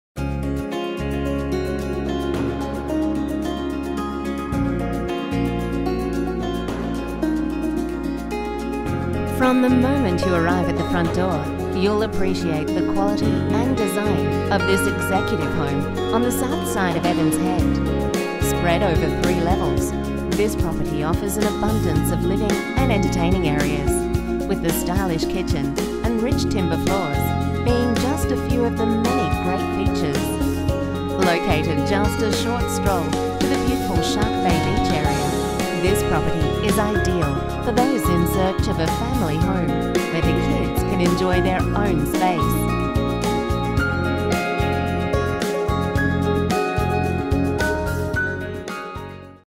5 PACIFIC CRESCENT
From the moment you arrive at the front door, you'll appreciate the quality and design of this executive home on the Southside of Evans Head.